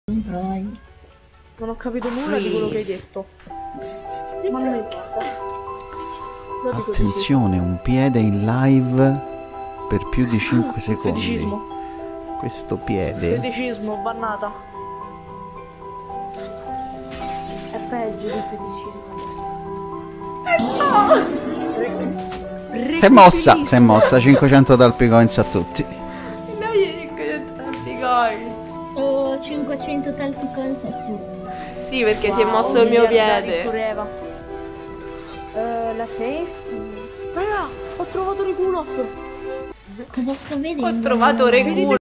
FUNNY FOOT FREEZE - Talpina freezata in live in posizione curiosa!
Ormai il Freeze è una tradizione delle live di Talpina. Stavolta è stata freezata in una posizione molto curiosa. Buona visione!